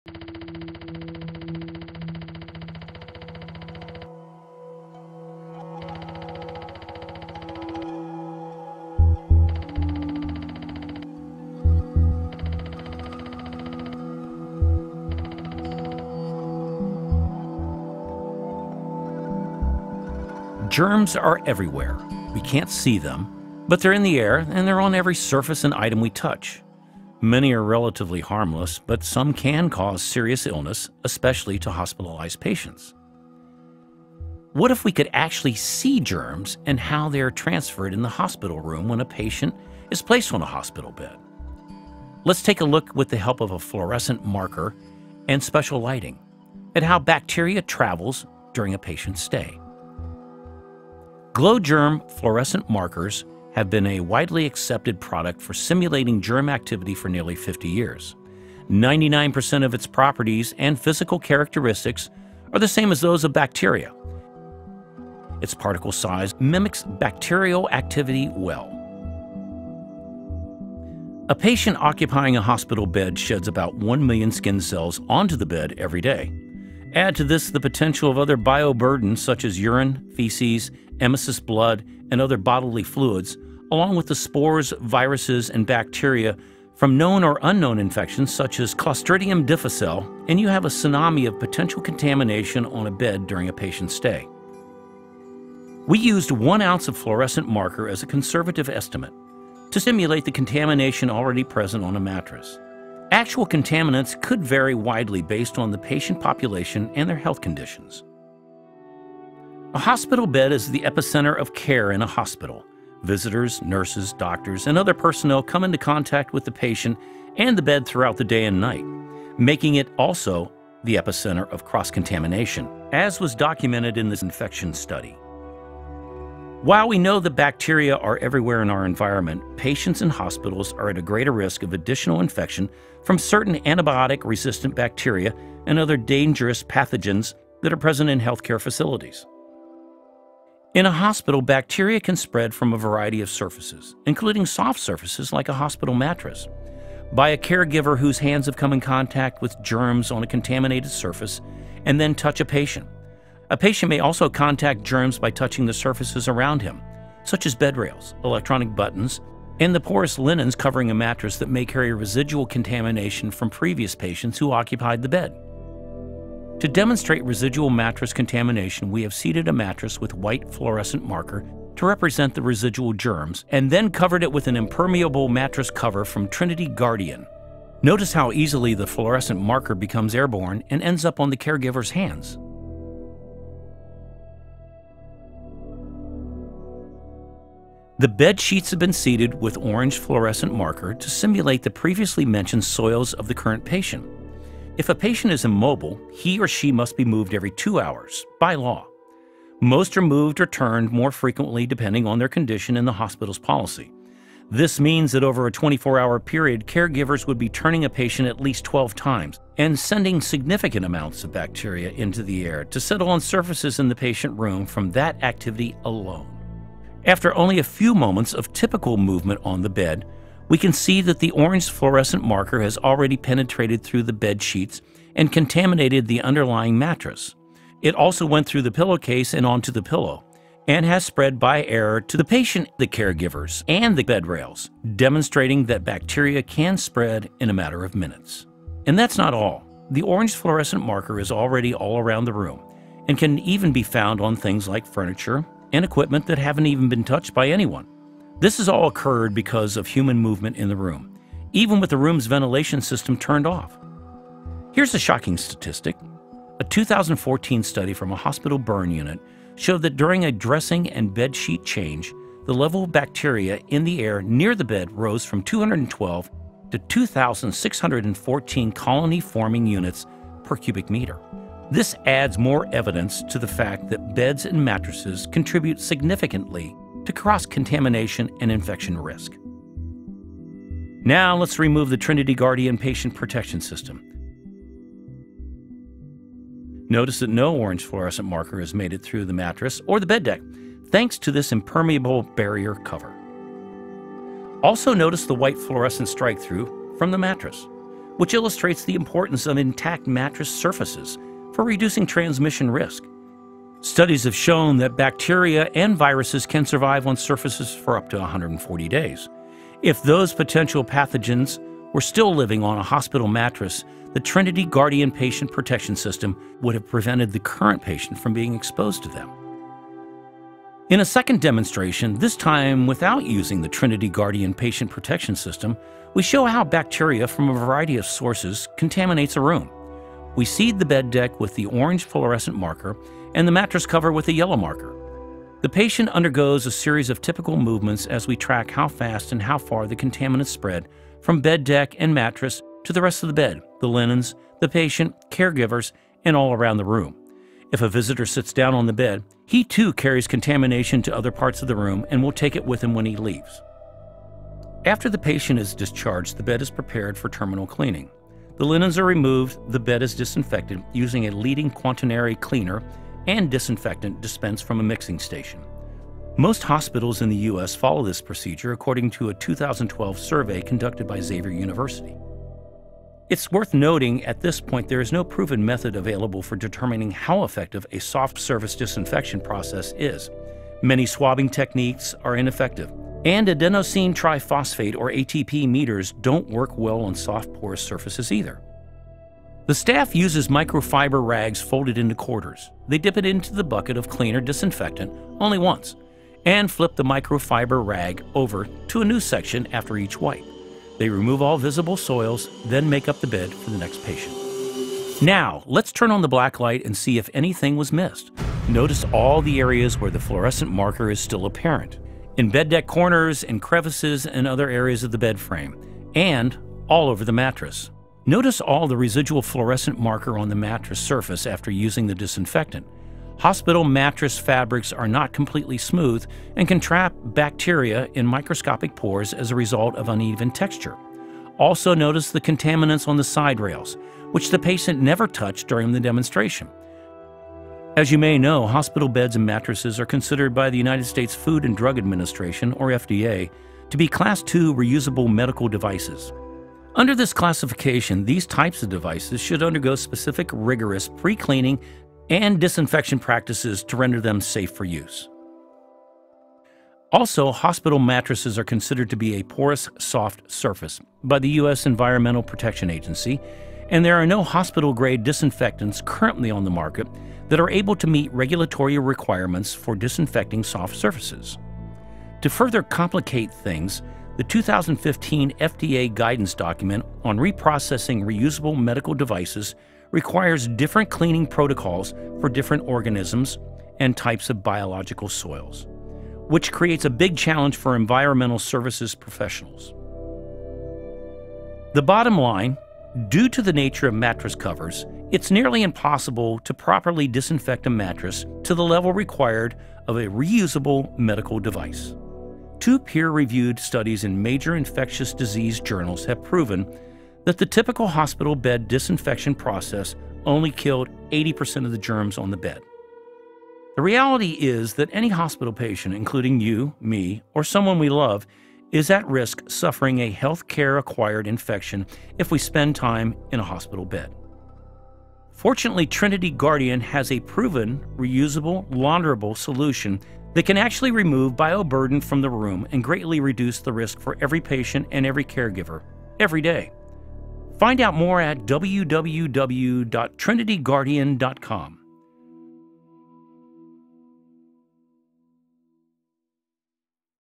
Glow Germ Video (full)
Video Objective: Demonstrate transfer of contamination in a patient room emanating from a hospital bed using fluorescent markers.